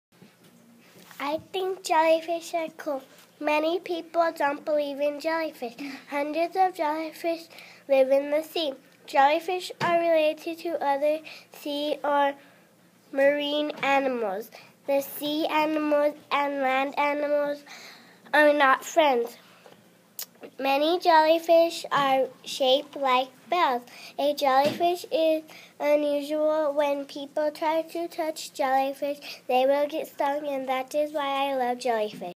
1st Grade Jellyfish Report
"B" reads her jellyfish report. We created this video using our Jefferson Impact iPads